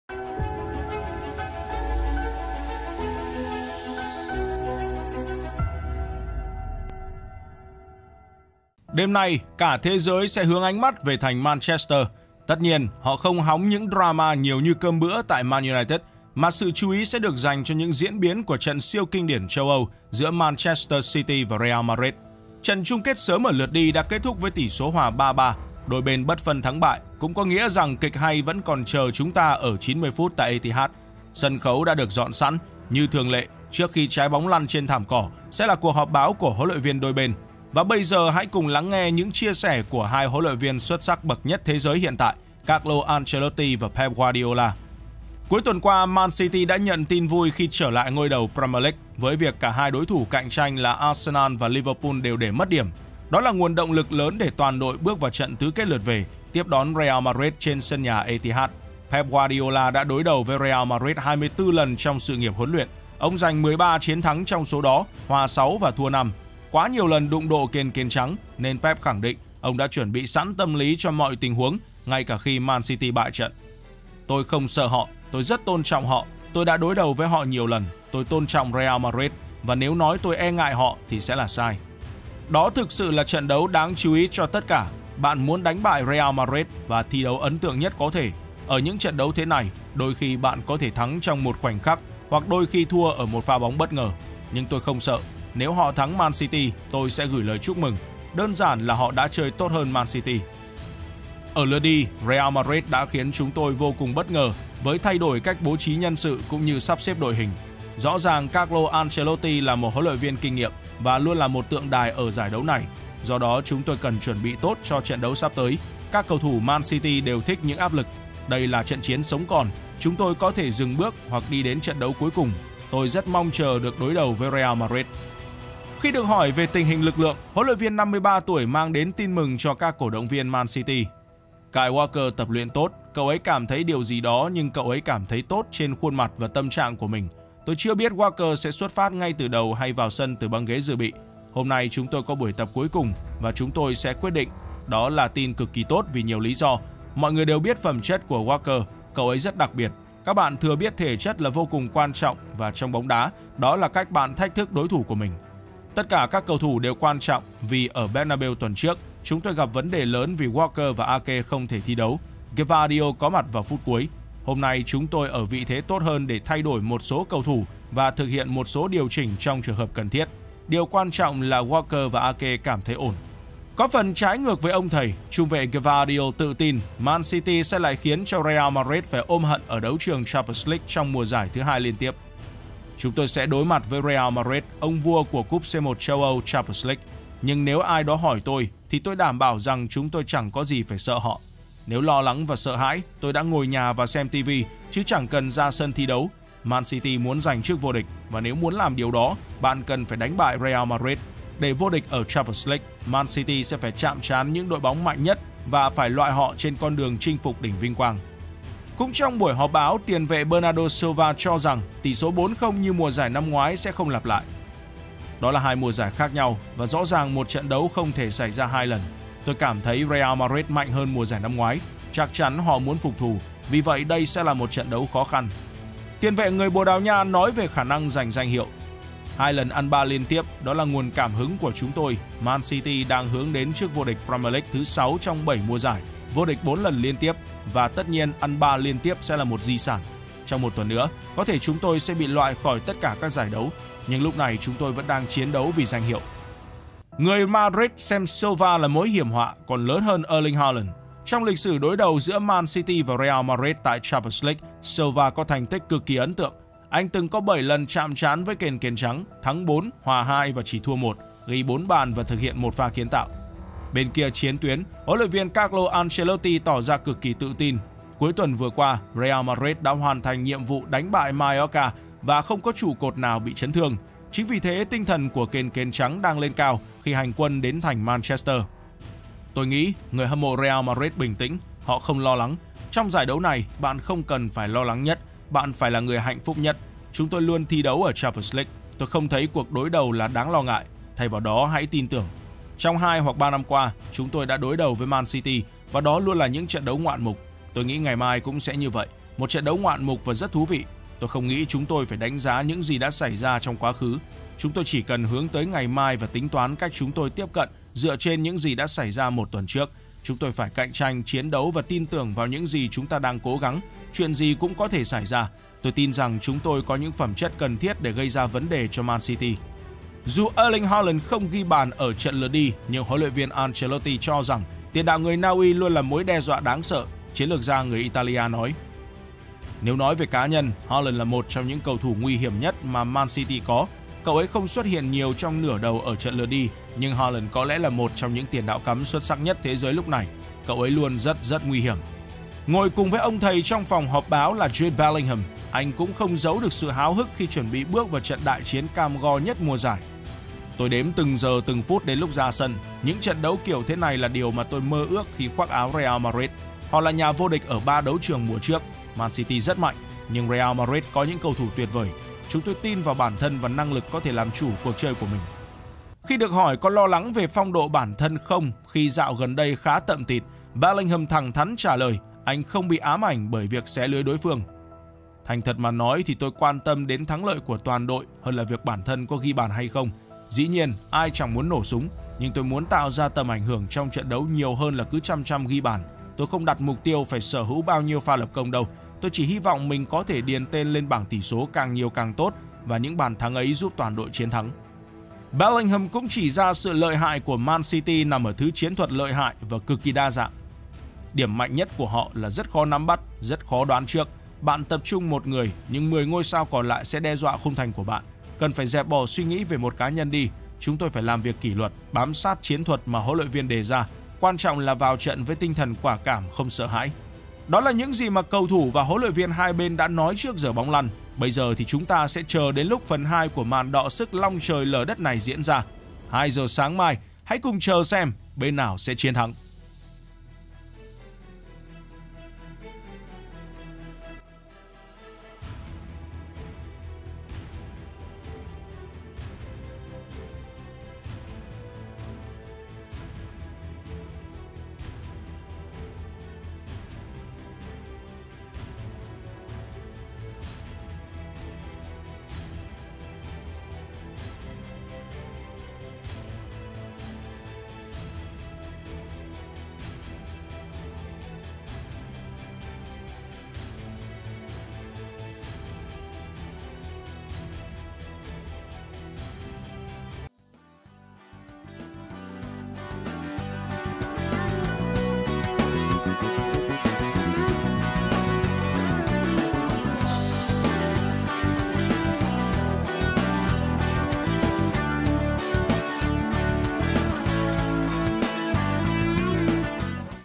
HỌP BÁO MAN CITY – REAL: ANCELOTTI TỰ TIN "XIN TIẾT" MC NGAY TẠI ETIHAD, PEP MUỐN XỬ ĐẸP KỀN KỀN
HỌP BÁO MAN CITY – REAL: PEP KHÔNG NGÁN ĐỐI THỦ, ANCELOTTI TỰ TIN CHIẾN THẮNG NGAY TẠI ETIHAD